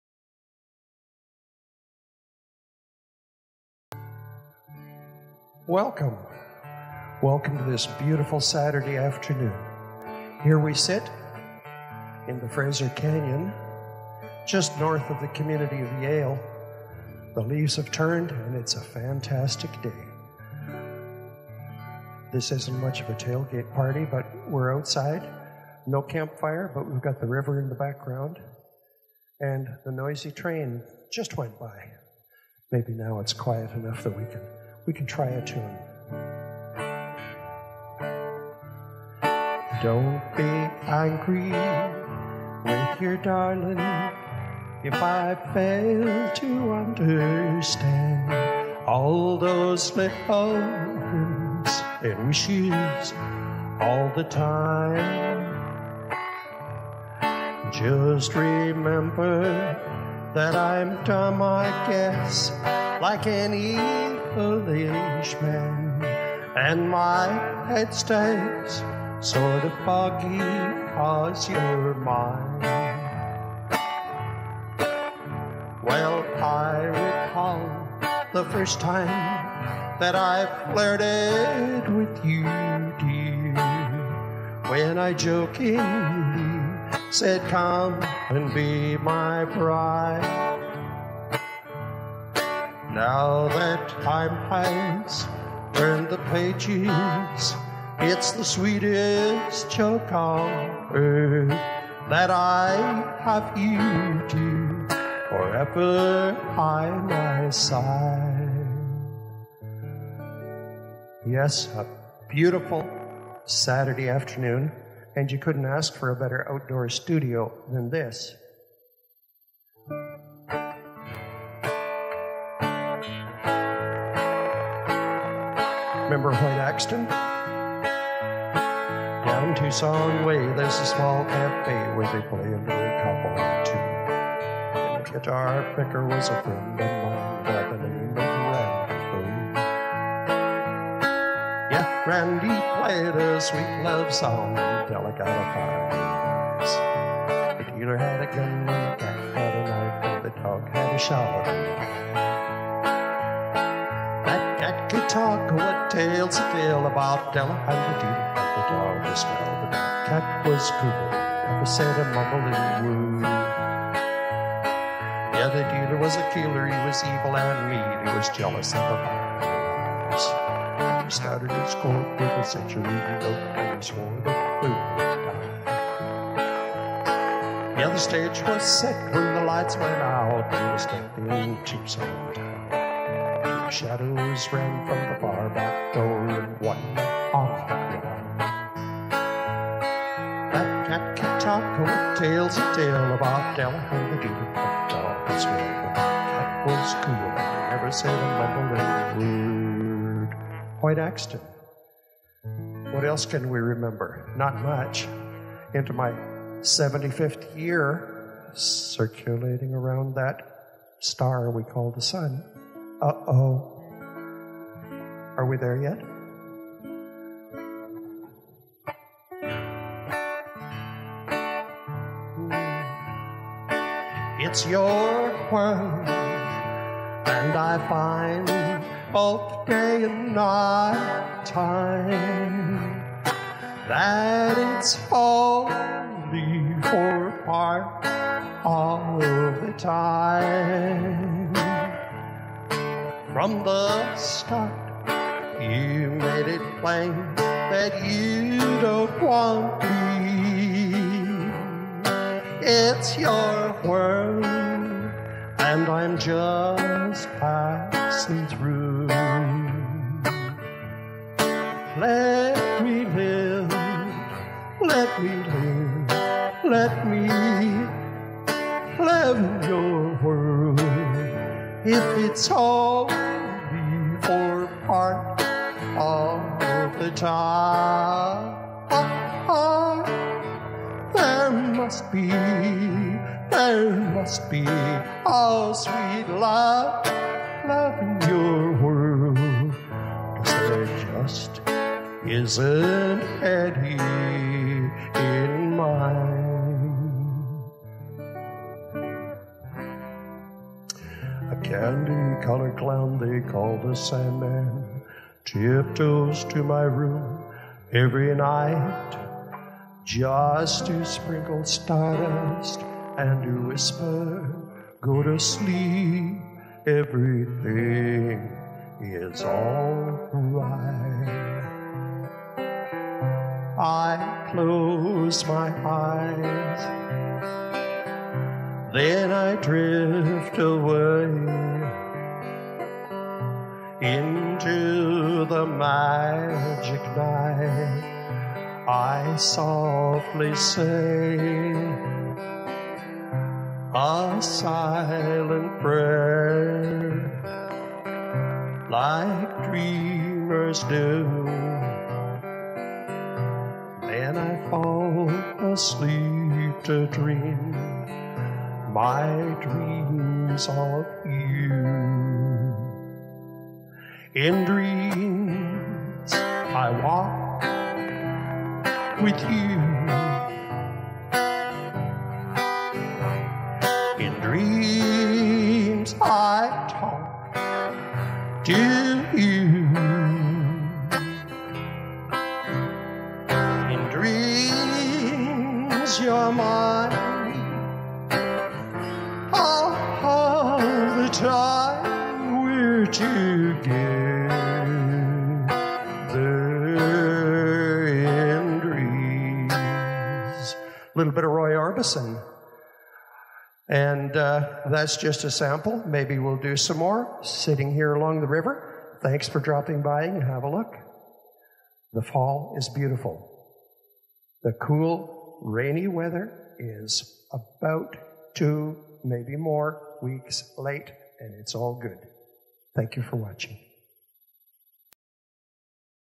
Music by the Fraser River | Classic Country Corner
Long over due! Enjoy...
A beautiful fall afternoon along the Fraser River near Franklin Rock.
I'm well into my 75th "Orbit around the Sun", by the end of August next year it's DONE!!! lol
.what's next?